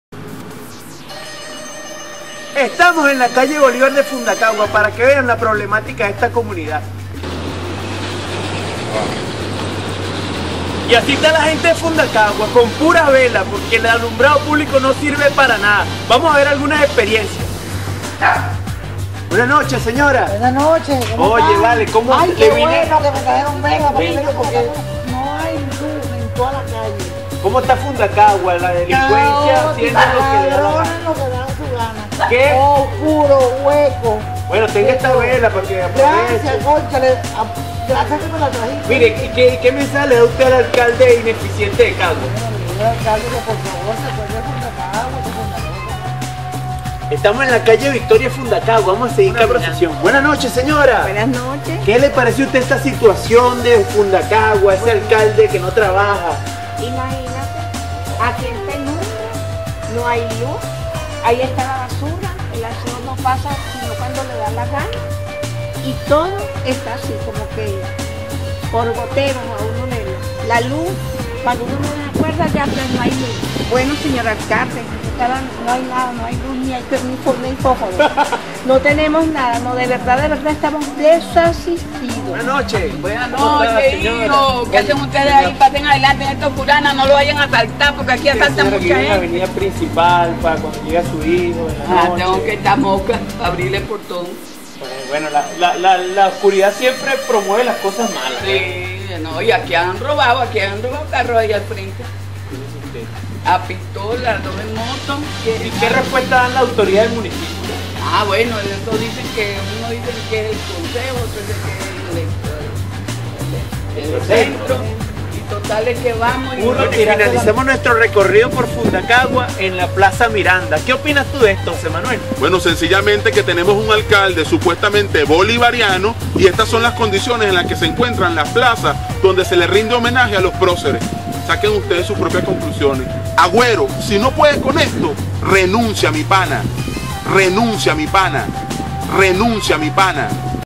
fundacagua en Tinieblas.
Transitar por las calles de fundacagua despues de las 6 pm es una mision imposible, el alumbrado publico es inexistente lo que se presta para que los delincuentes hagan de las suyas.